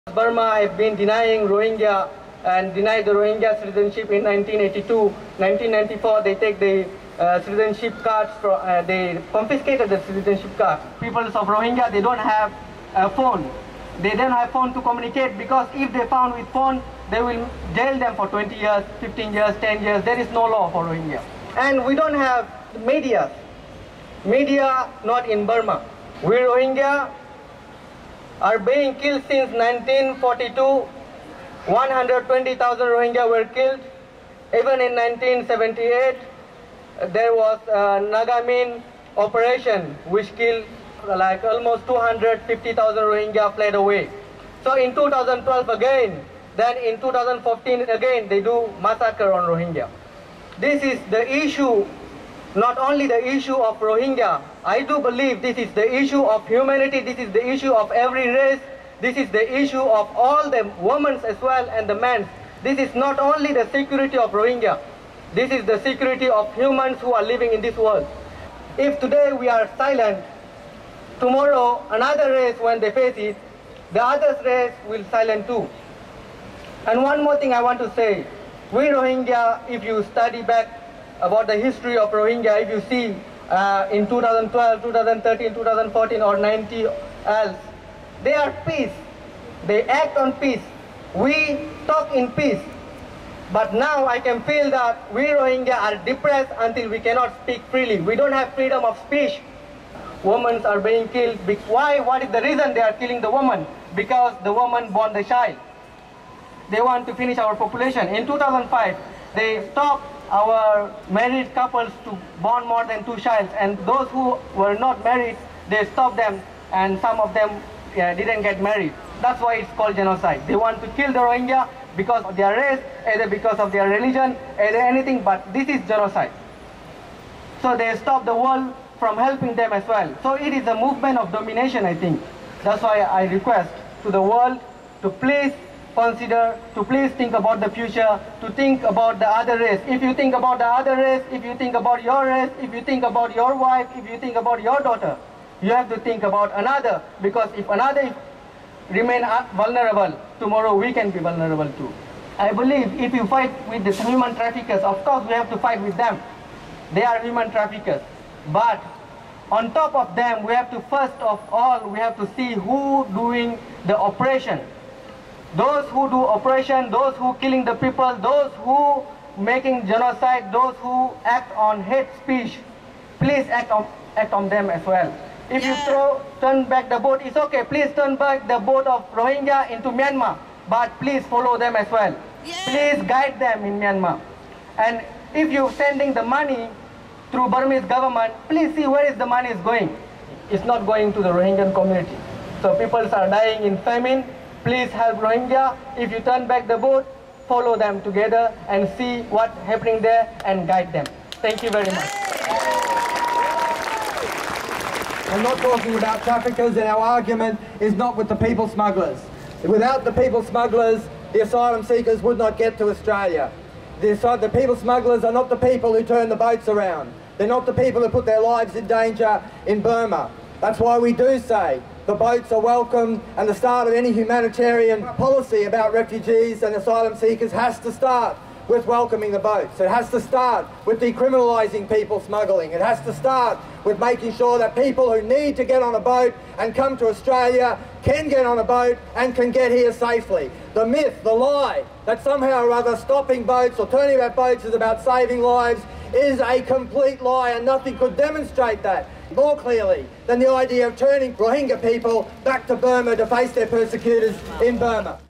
ROHINGYA APPEAL TO AUSTRALIA
I went to listen to the ROHINGYAN Appeal tonight in Sydney. They've been stripped of citizenship, driven off the land and killed by the hundreds of thousands since 1942. They are starved, reproduction is curbed and they are not allowed to own a phone.